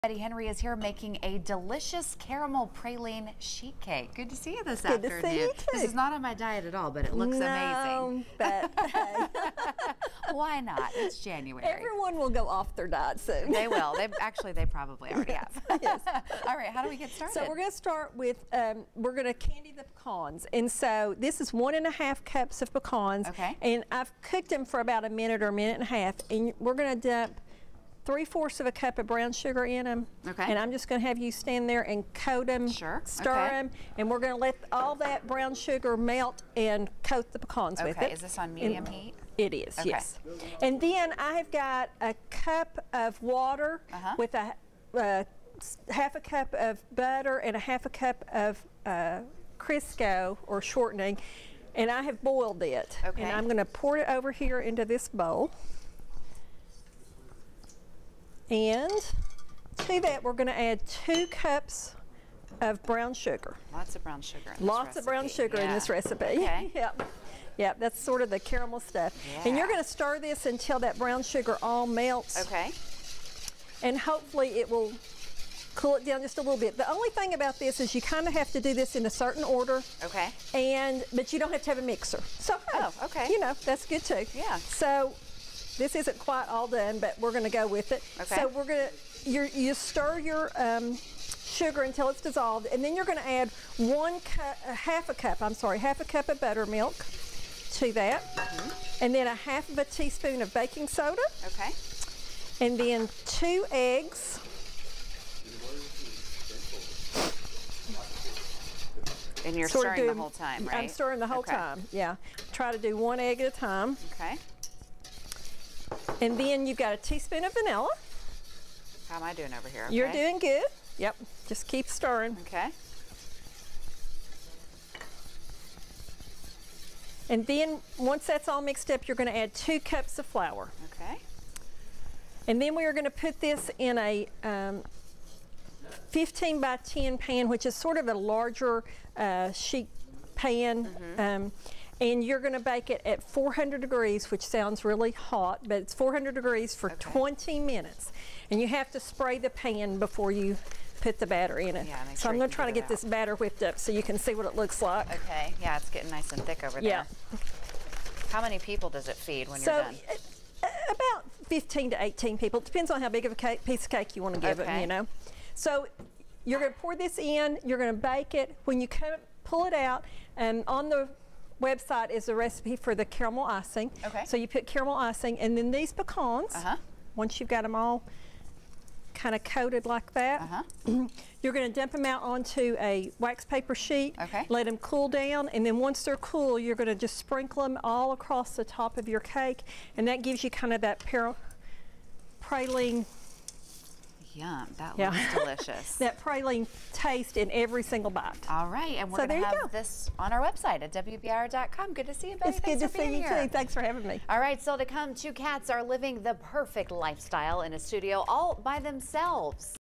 In the kitchen: Caramel praline sheet cake
Betty Henry is here making a delicious caramel praline sheet cake!